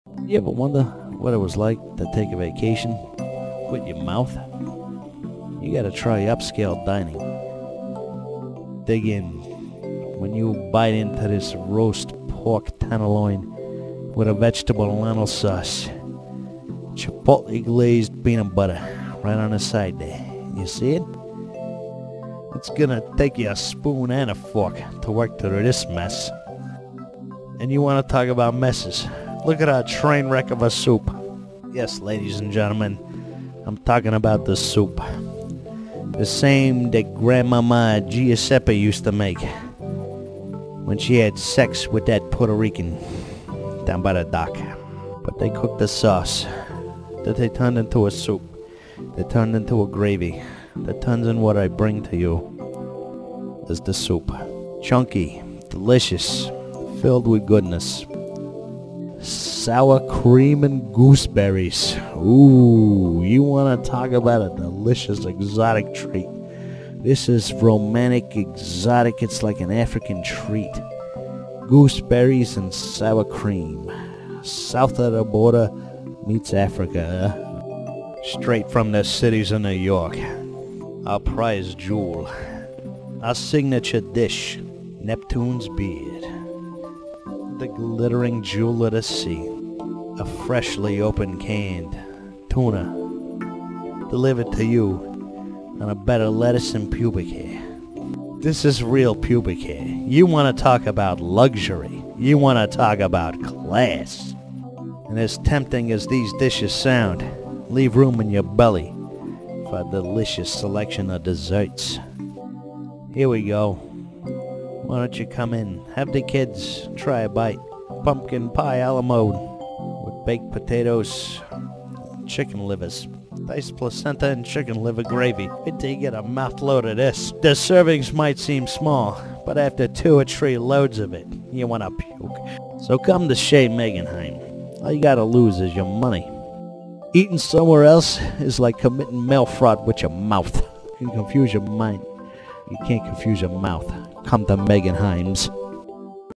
Chez Megenheim's Alarming Cuisine (raw improv for PROGRAM 2)
Completely raw material for the upcoming PROGRAM 2 - Mr. Ted Megenheim expounds over shaky handheld footage over his many delicacies, delights, and diarrhearetic  combos.